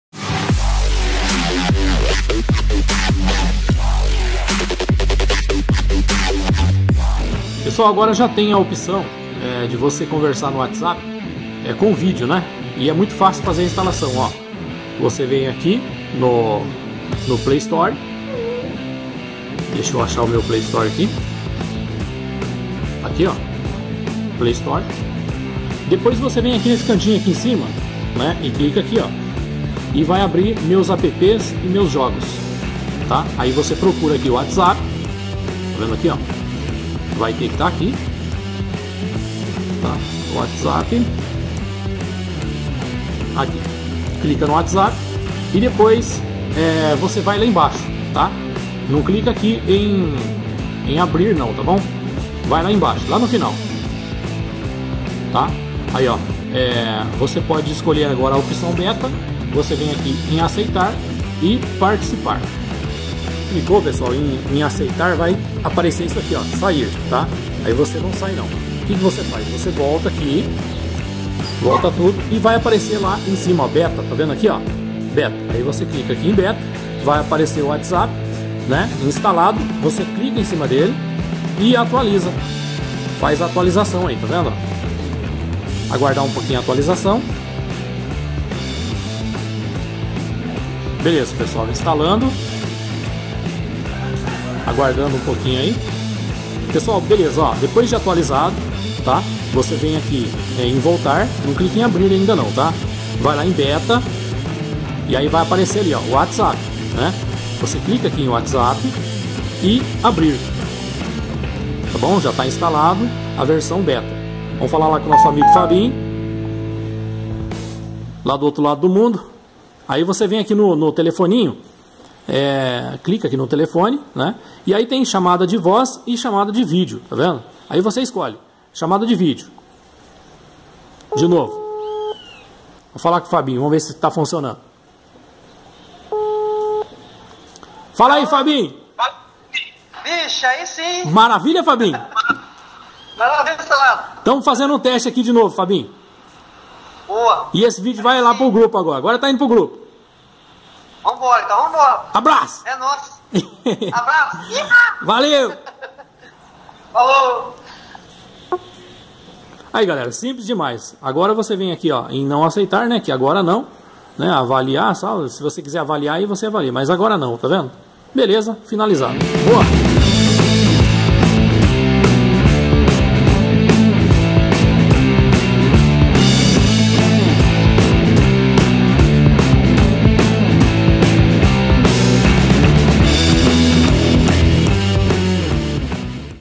SÓ PRA AJUDAR A GALERA DO CANAL WHATSAPP
Galera esse video é só pra ajudar as pessoas que ainda não sabem,
agora o Whatsapp disponibilizou a chamada de video e é muito fácil a instalação confira nesse video e um grande abraço do Saladinha.